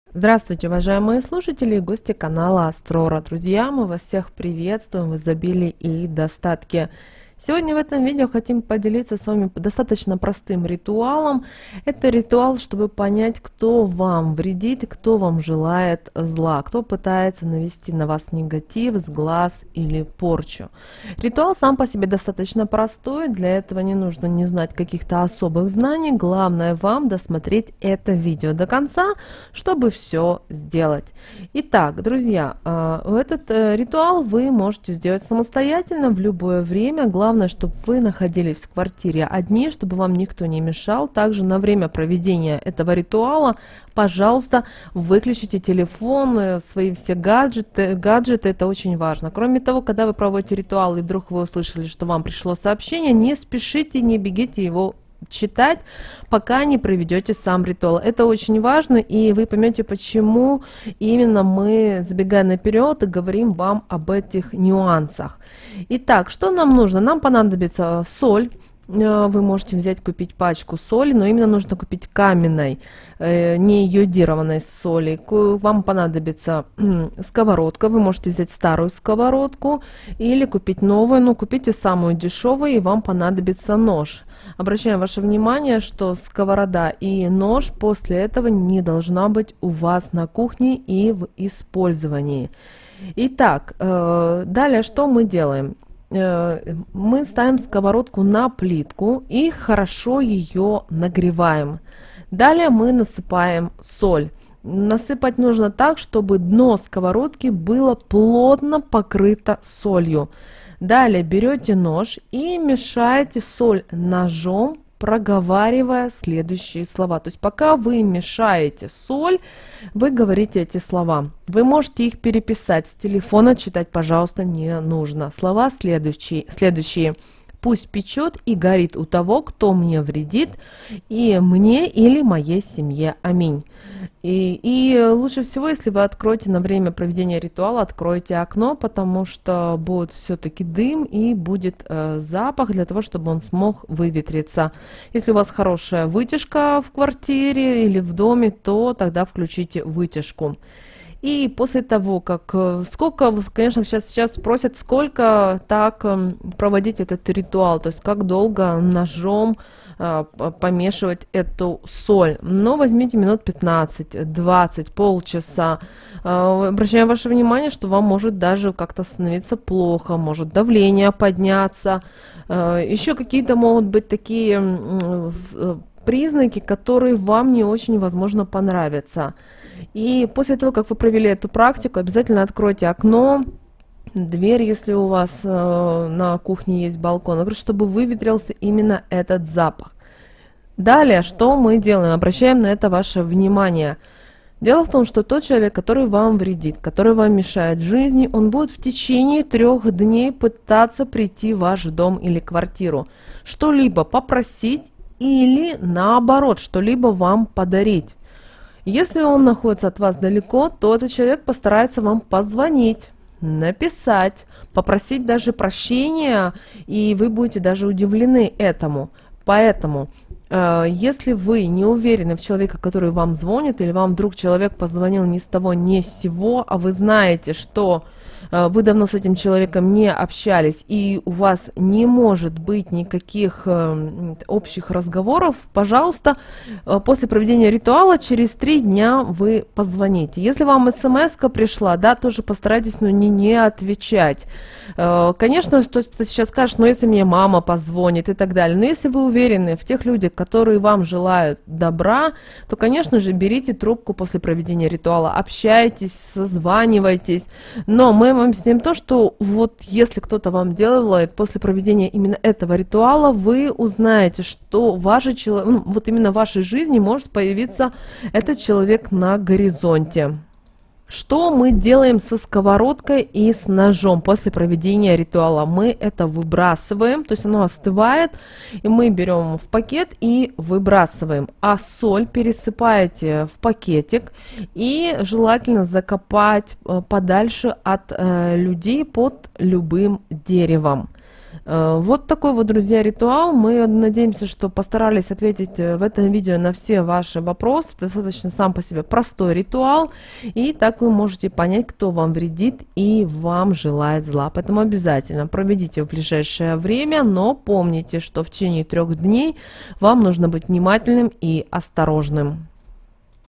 Узнайте кто вам вредит и желает зла в жизни, через три дня вы удивитесь результату
Простой ритуал поможет Вам узнать, кто Вам вредит, кто желает зла. Завистников много, но не всегда знаешь, кто это может быть. Ритуал простой и действенный.

"Пусть печёт и горит у того, кто мне вредит мне или моей семье. Аминь"